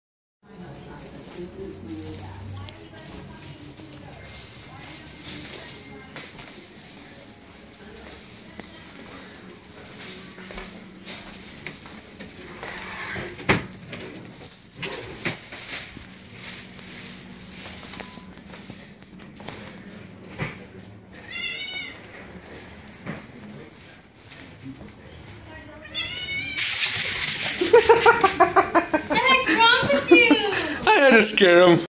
Scaring my 6 month old cat with compressed air
Spraying compressed air at my cat to scare him. No animals were harmed in the making of this film.